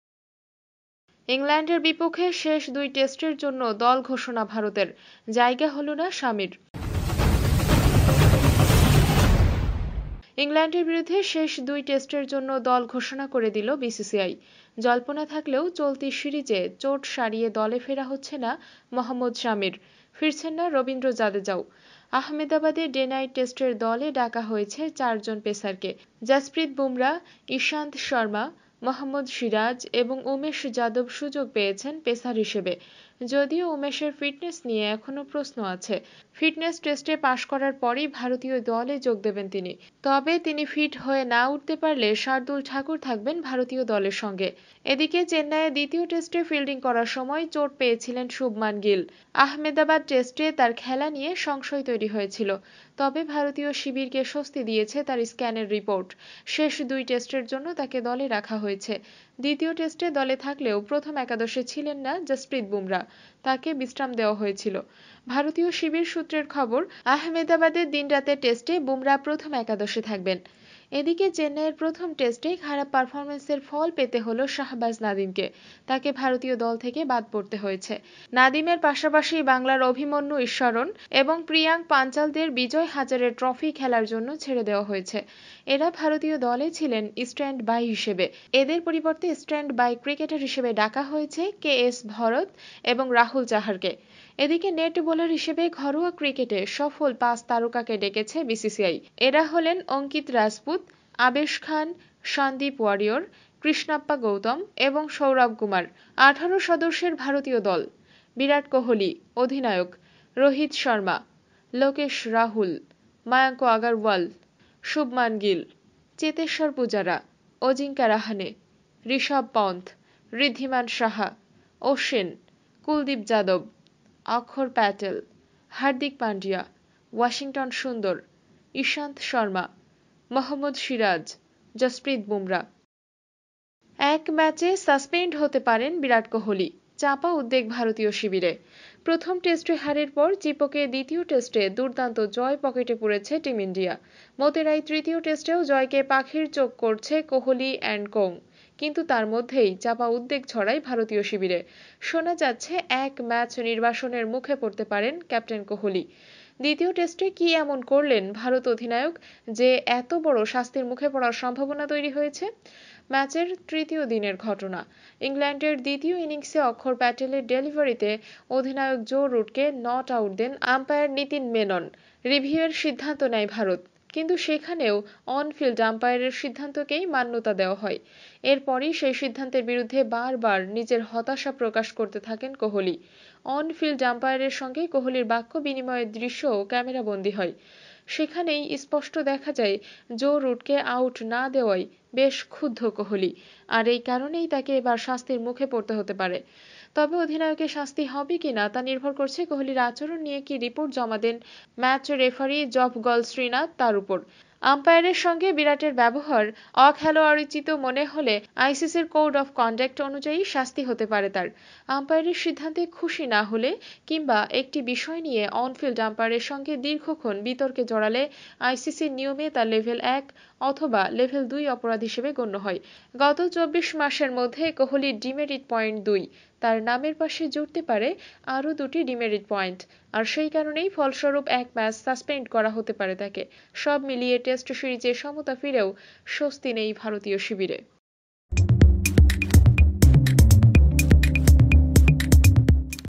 ইংল্যান্ডের বিপক্ষে শেষ দুই টেস্টের জন্য দল ঘোষণা ভারতের || বড় ধরনের শাস্তি হতে পারে কোহলির | India
ইংল্যান্ডের বিপক্ষে শেষ দুই টেস্টের জন্য দল ঘোষণা ভারতের, জায়গা হল না শামির; বড়সড়ো শাস্তি হতে পারে কোহলির; IND vs ENG 2021